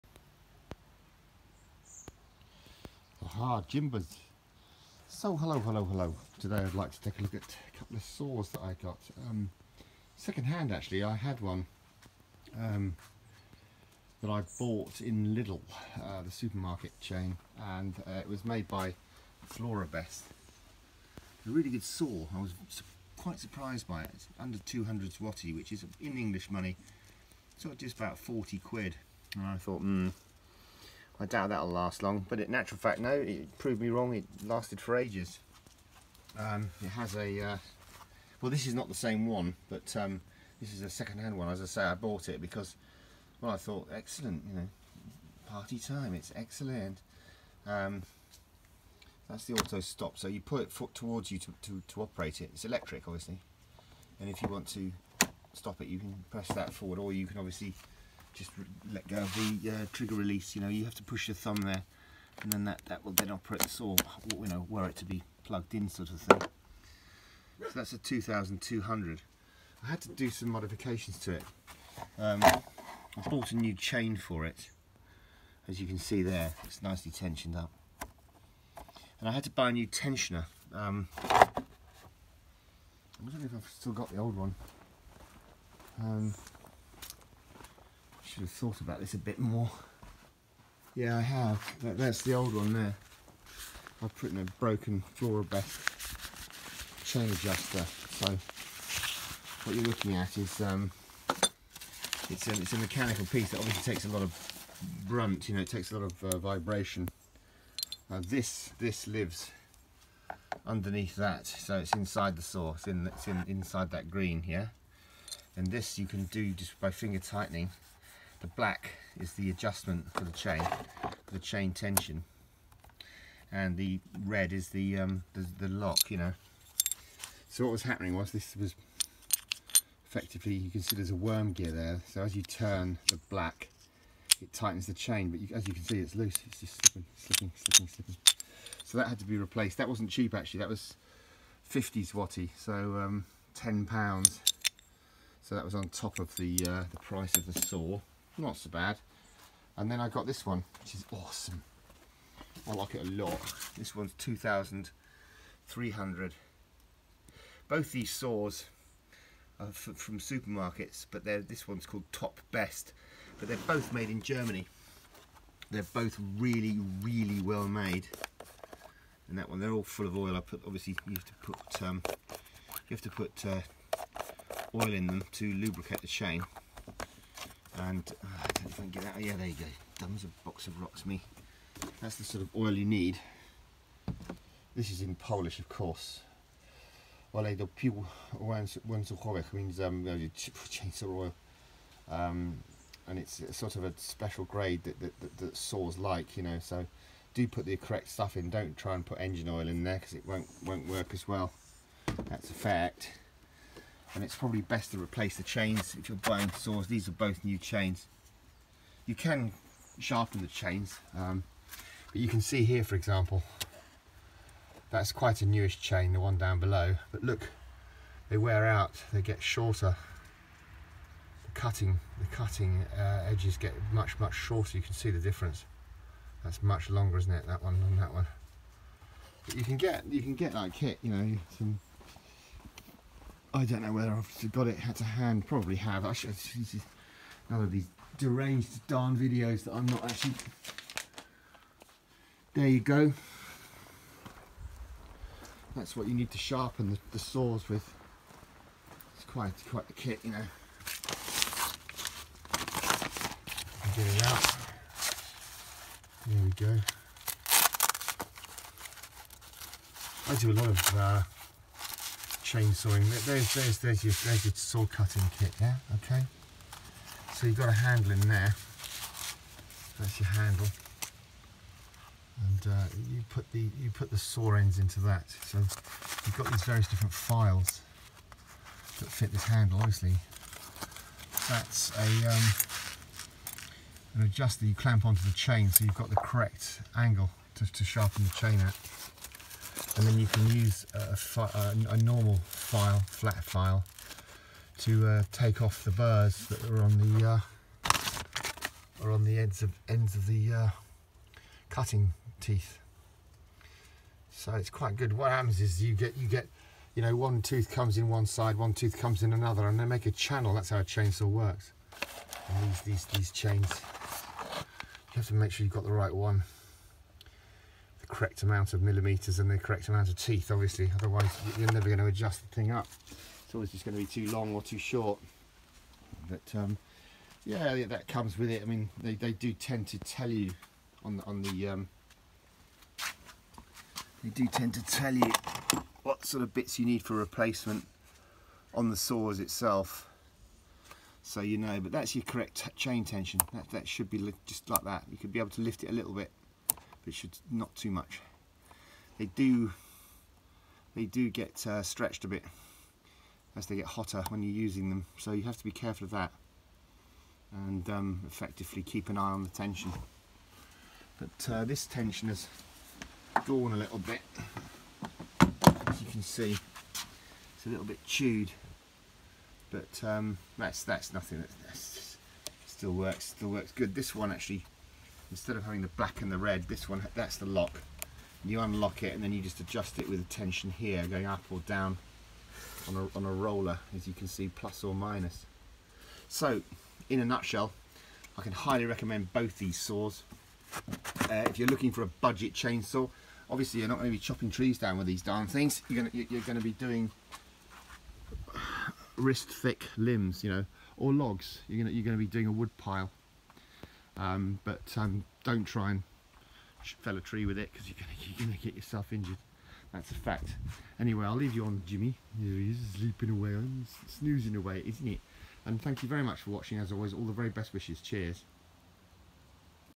A tale of two chainsaws
I recently replaced my Florabest FKS 2200W electric chainsaw with another second hand model. New chain and tensioner and the old girl works like a dream. Whilst I was searching for the new parts I noticed a Top Craft 2300W saw for sale that I had parts for on hand to restore.. so now I have two !!
These budget supermarket saws are perfect for work around the garden.. but please be aware that they are not powerful enough to fell timber.